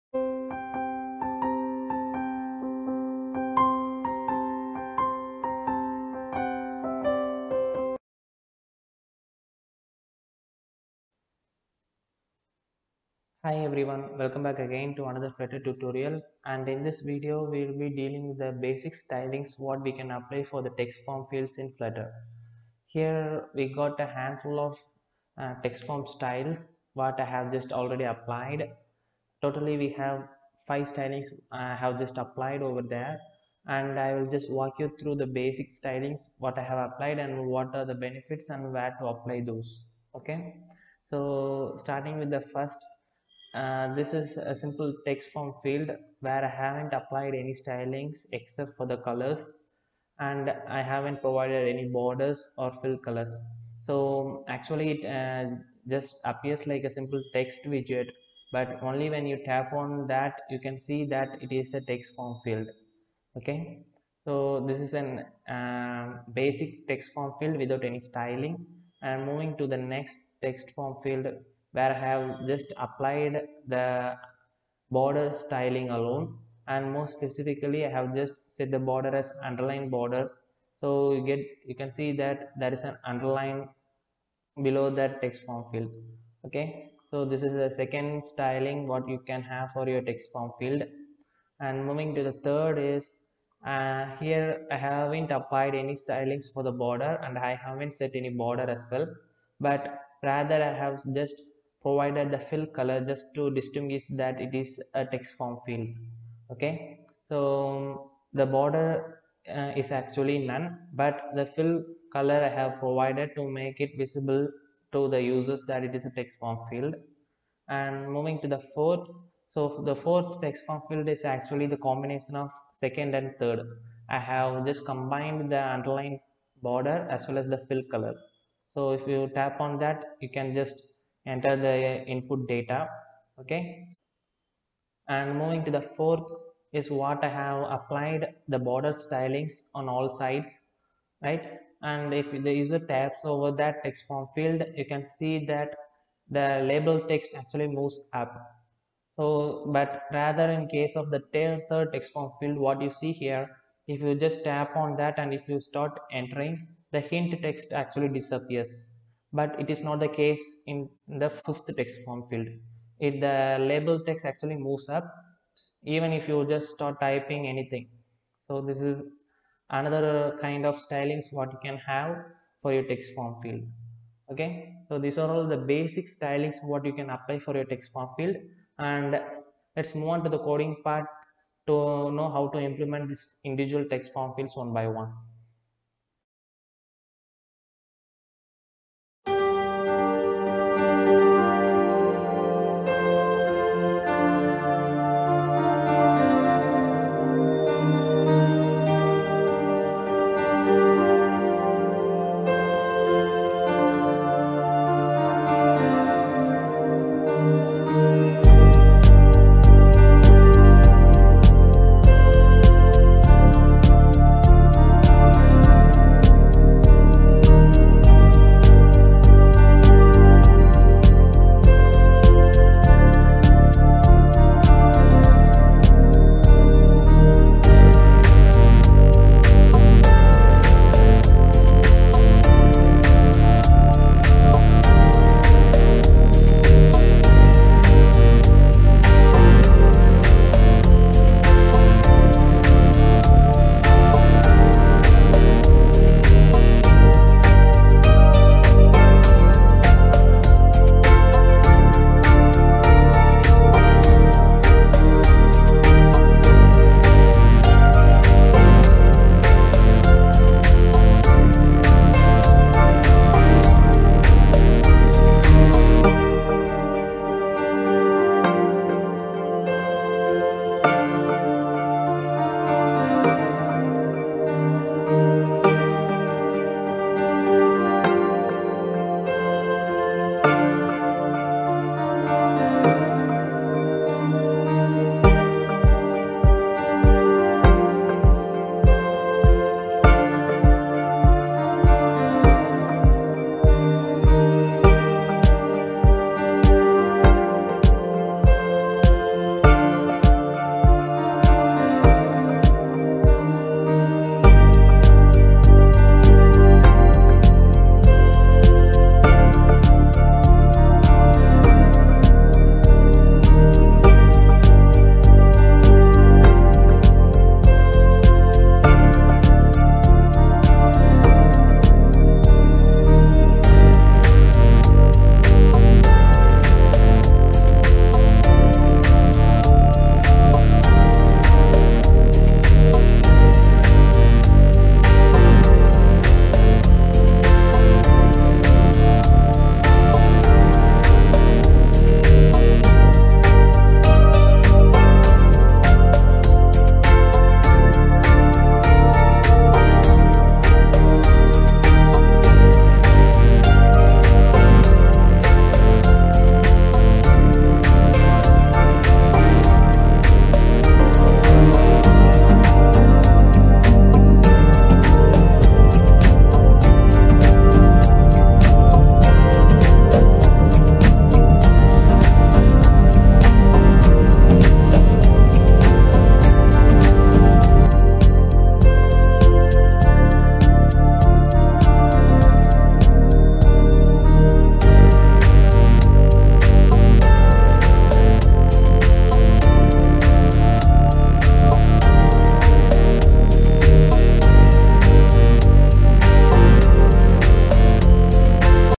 Flutter TextFormField Stylings - Basics of text form styles in Flutter | Apply styles to TextField.
Style your TextFormField Widget in Flutter